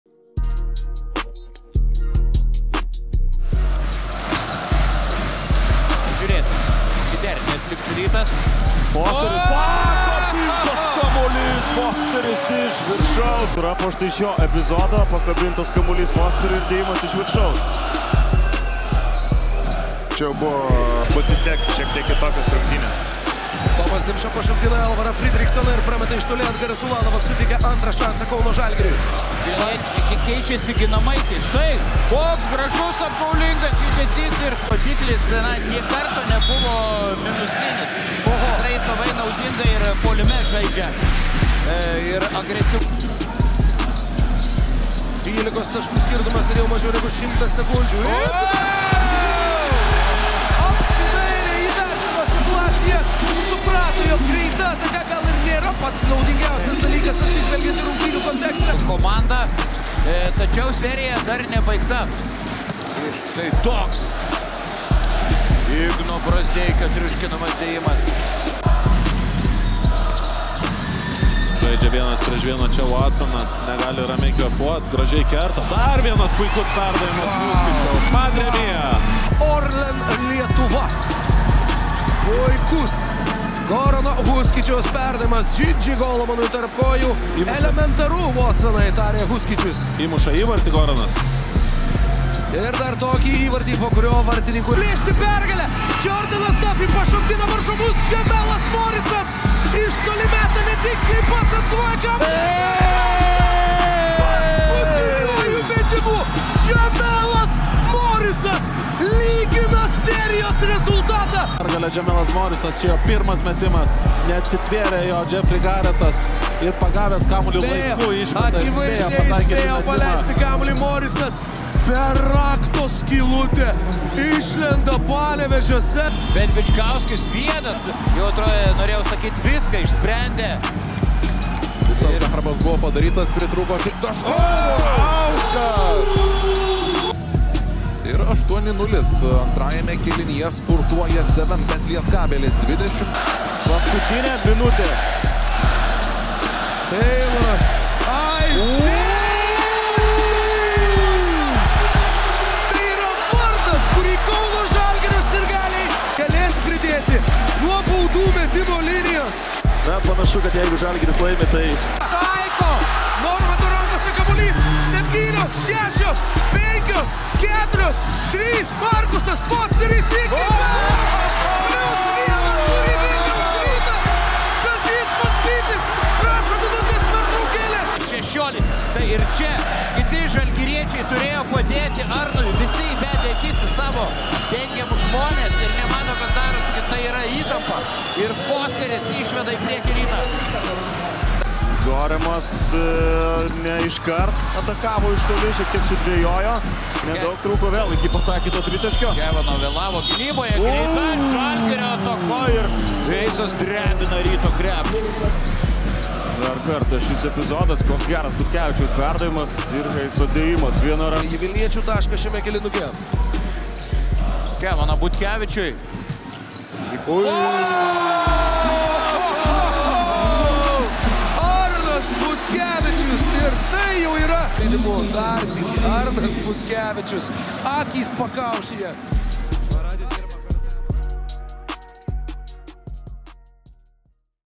„Betsafe-LKL“ TOP 10 | Birželis
Praskriejo paskutinysis jubiliejinio „Betsafe-LKL“ sezono mėnesis, kuriame netrūko staigmenų, įspūdingų dėjimų, gražių perdavimų ir lemiamų paskutinės sekundės metimų.

Kviečiame jus pasigrožėti įspūdingiausiais birželio mėnesio „Betsafe-LKL“ mėnesio epizodais.